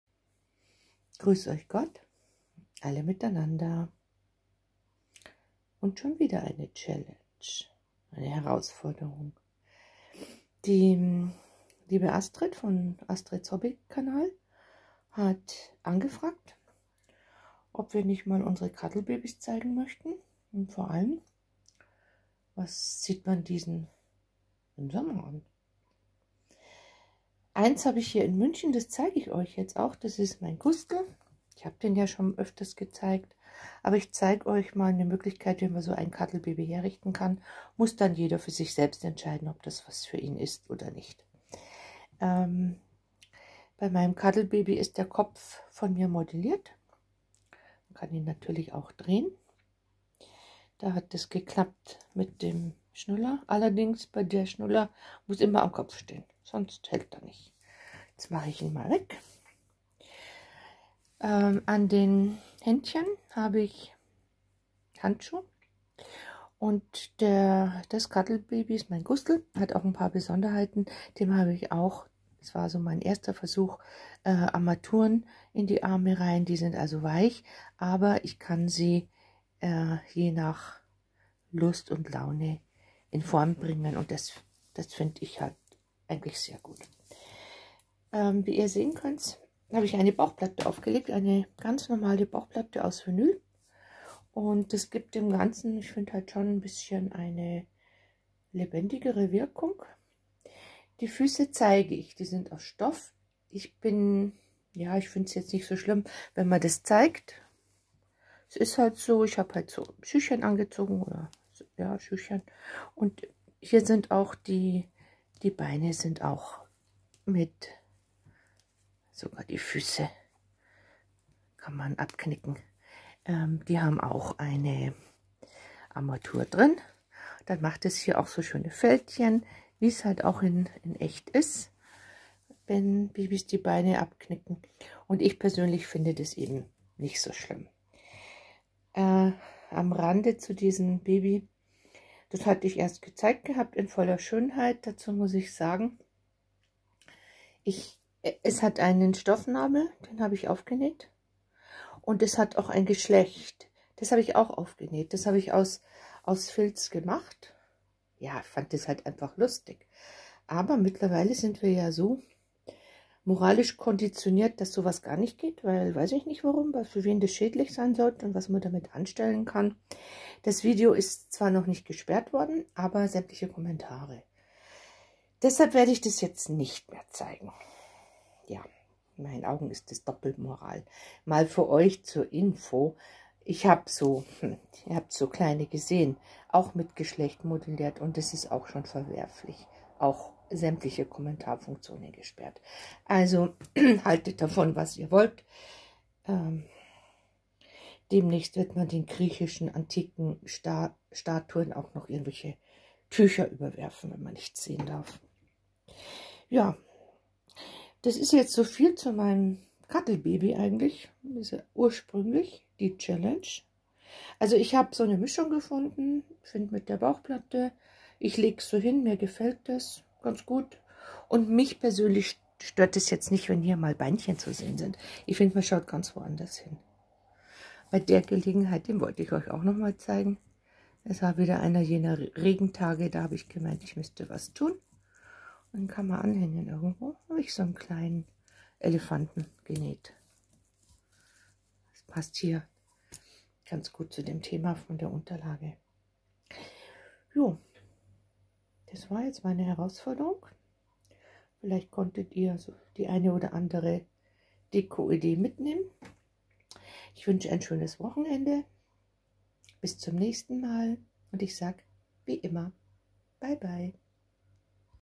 Zipfelmützchen OOAK Baby ♥CHALLENGE - Cuddle Baby Kleidung im Sommer♥
viel Spaß mit meinem Bekleidungs-Vorschlag für ein Voll-Cuddle-Baby an heißen Sommertagen!